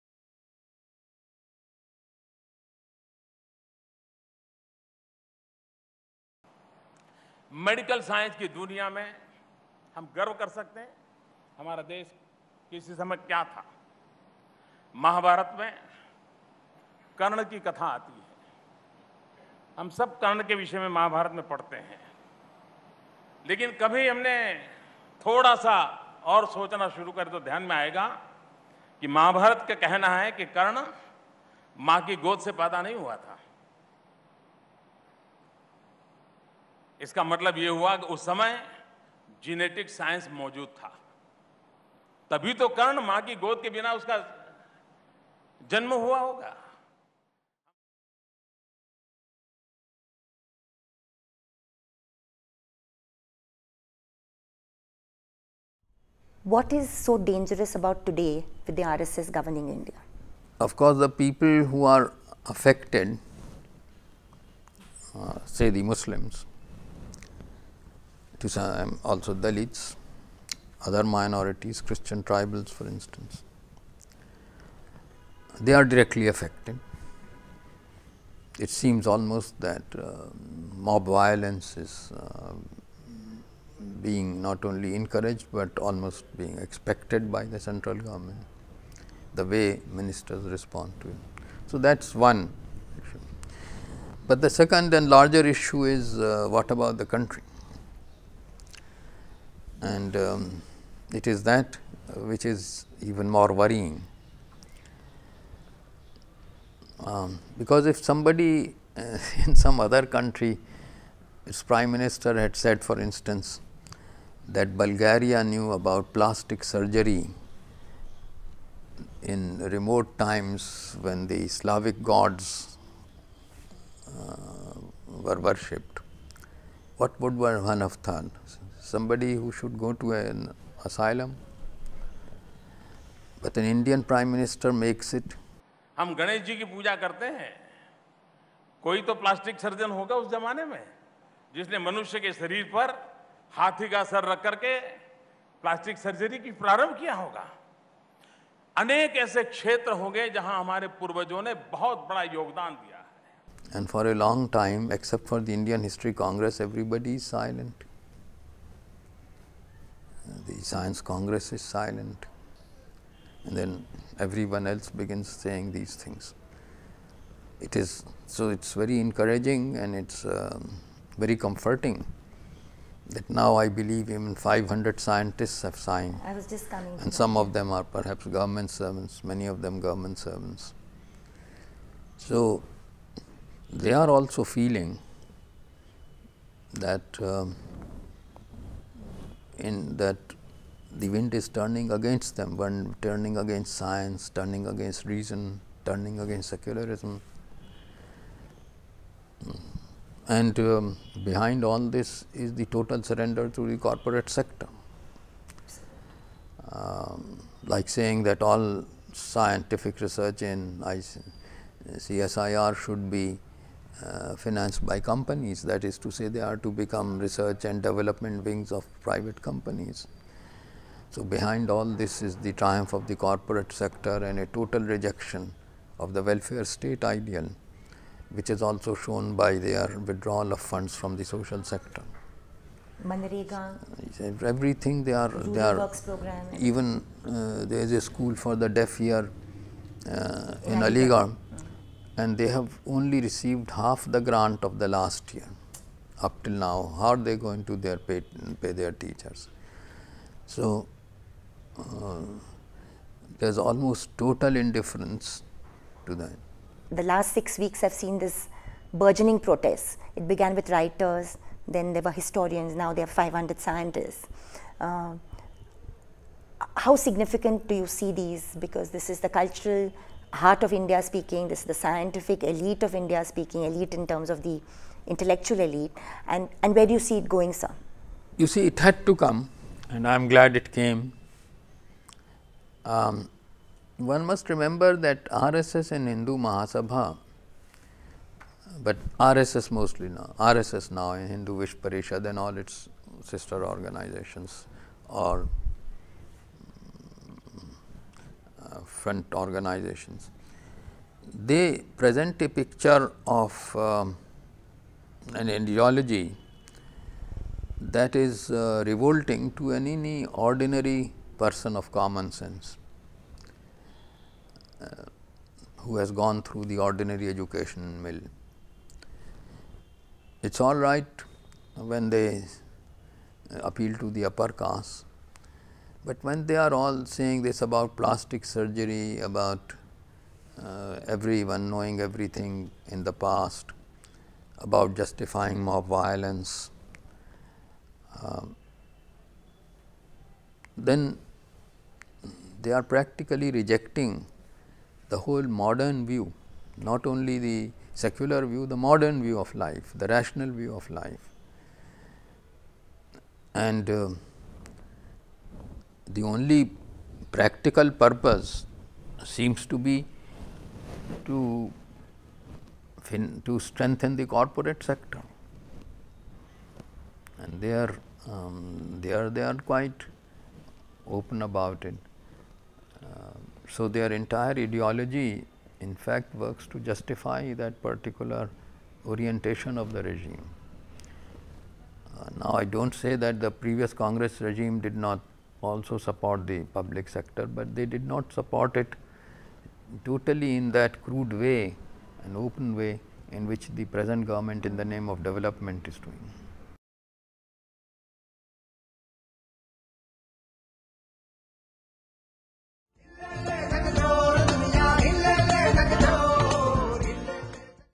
RSS is against Science and Reason
Eminent historian Irfan Habib and Teesta Setalvad discuss why the current RSS government is dangerous for India.